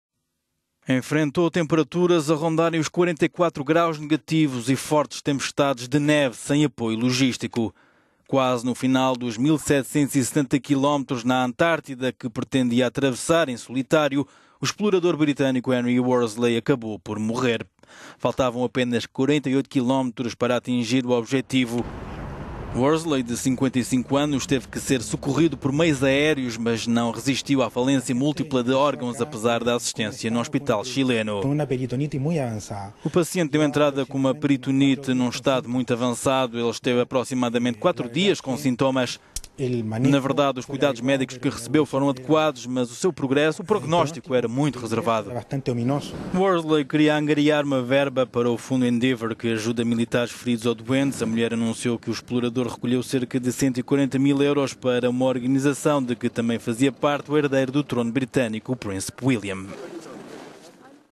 Explorador solitário morre no final da travessia da Antartida
Enfrentou temperaturas a rondarem os 44 graus negativos e fortes tempestades de neve sem apoio logístico. Quase no final dos 1770 quilómetros na Antártida que pretendia percorrer, o explorador britânico Henry Worsley acabou por morrer.

Faltavam apenas 48 quilómetros para atingir o objetivo, que era reproduzir uma expedição iniciada há um século por outro britânico.

Worsley, de 55 anos teve que ser socorrido por meios aéreos, mas não resistiu à falência múltipla de órgãos, apesar da assistência…